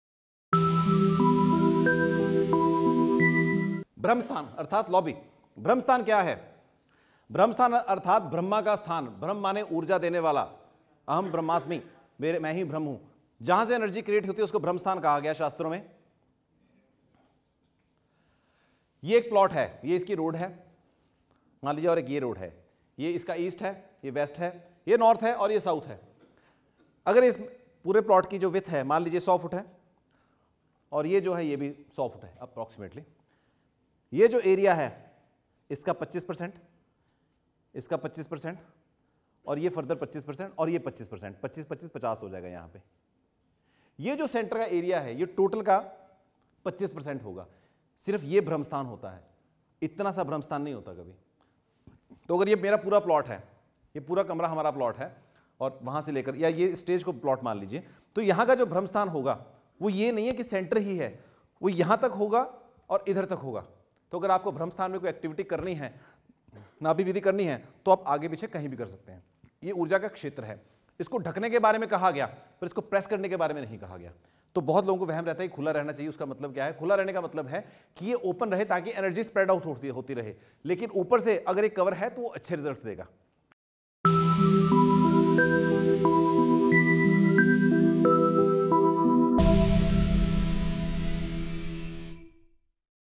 What is Brahamsthan and Myths about it
Vastu tips for Brahamsthan in House and remedies for the error zones

Effect of Brahamsthan in East, ESE, SE, SSE, South, SSW, SW, WSW, West, WNW, NW, NNW, North, NNE, NE, ENE

Balance the Brahamsthan by doing Nabhi Vidhi there and live in energetic house

Rahul Kaushl
Occult Master & Celebrity Astrologer

- Pandit.com
- FreeKundli.com
- OccultMaster.com